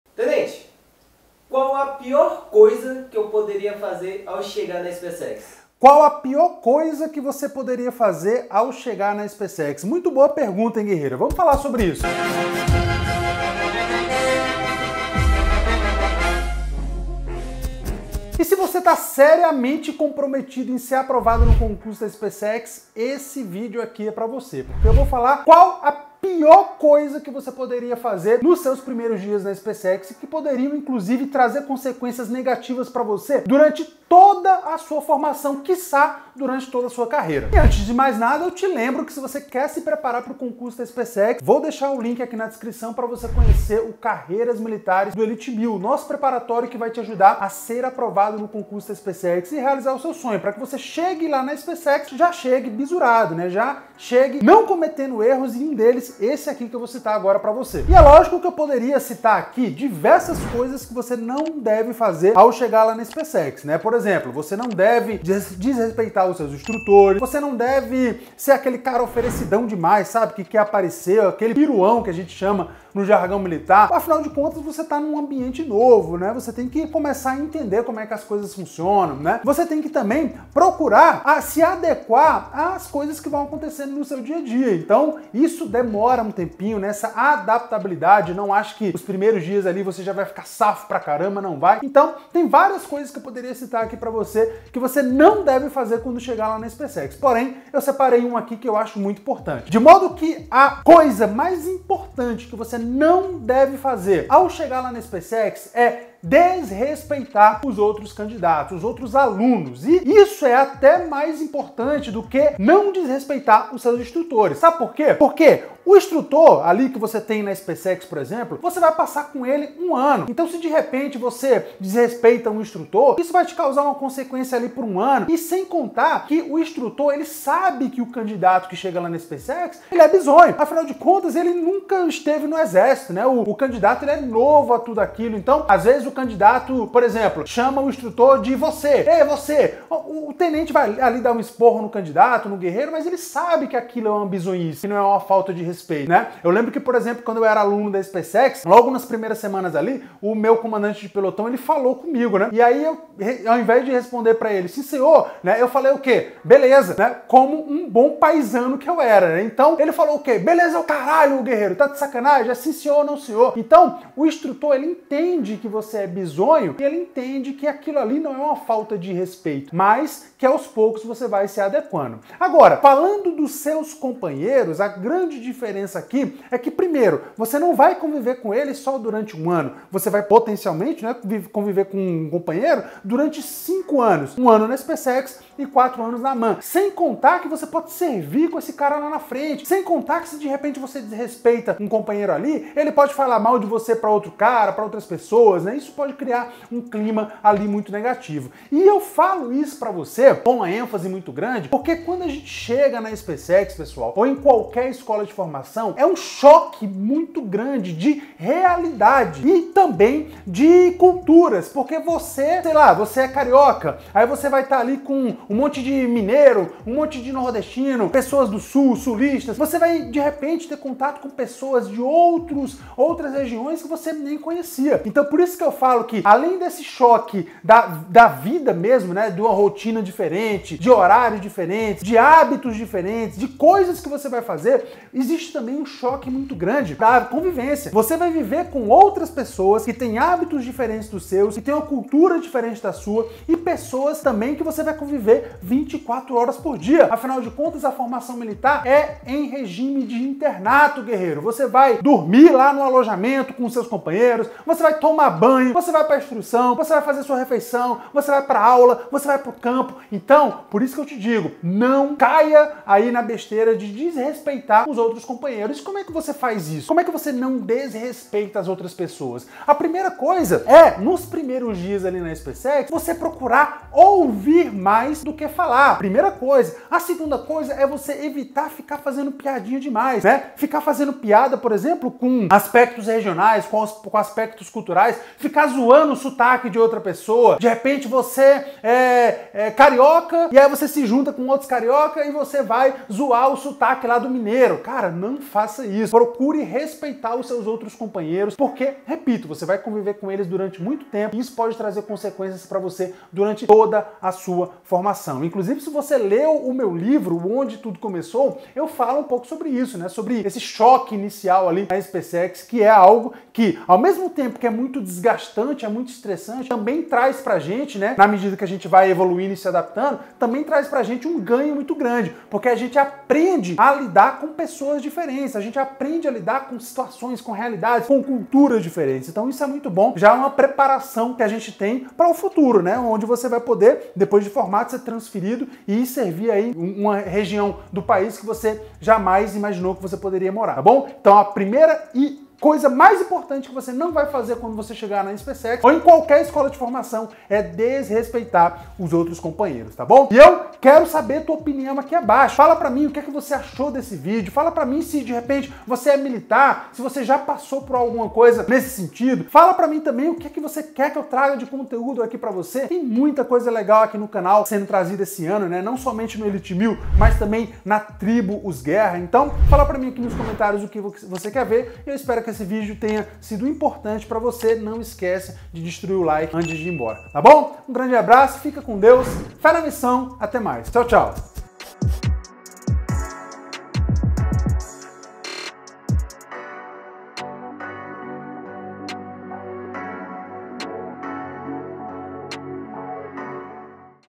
A PIOR COISA QUE VOCÊ PODE FAZER AO CHEGAR NA ESPCEX
Existem inúmeras coisas que você precisa se atentar em não fazer, mas acredito que essa é uma das mais importantes!

🪖 Mentoria MENTE DE AÇO: Encontre-se semanalmente com o Ten. Thiago Henrique e o Capitão Danilo Cacavo, e construa uma mente inabalável para ser aprovado no concurso militar -